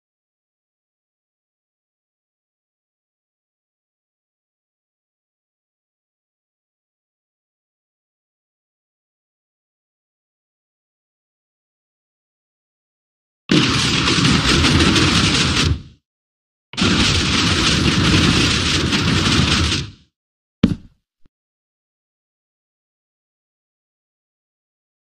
Video for the bow tie man 5x5 vault cave door PS: it is java
this is a video for the bow tie man